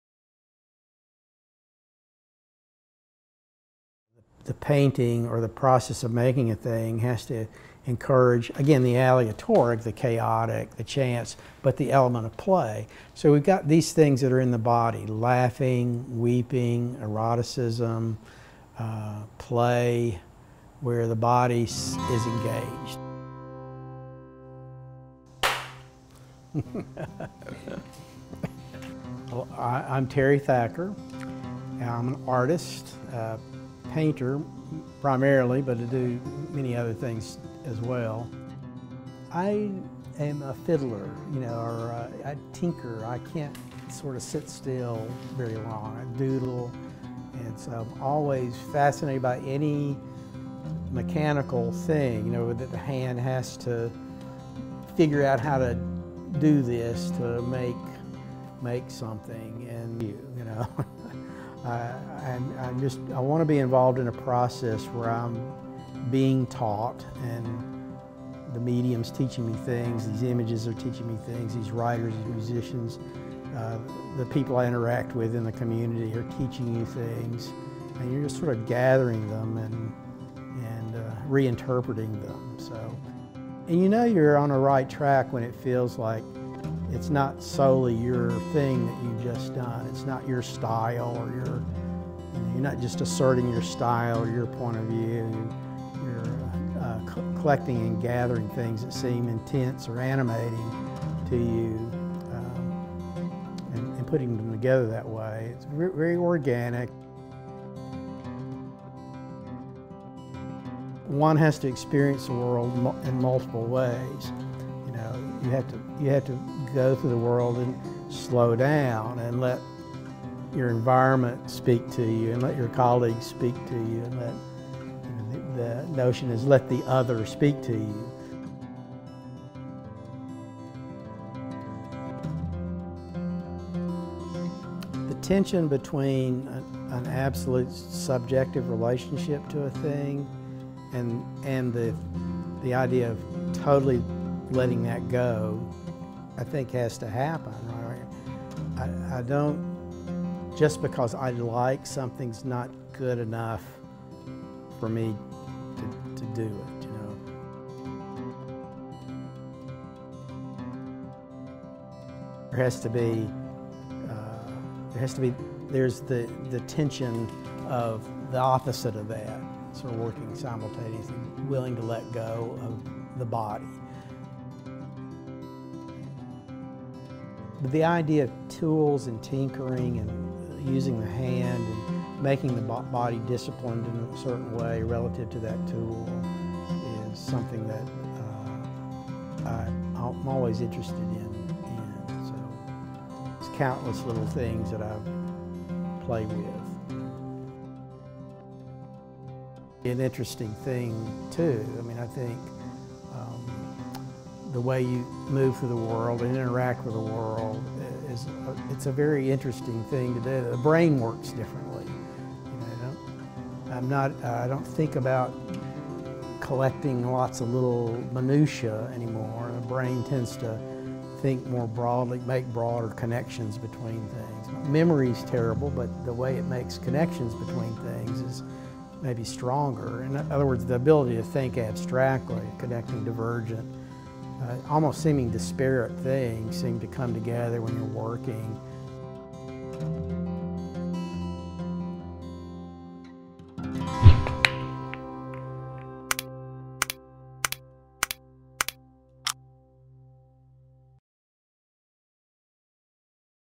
Open Studio: Terry Thacker
At home and up front, visual artist and Watkins professor Terry Thacker talks process, aesthetics, and how restlessness has helped him create a body of work.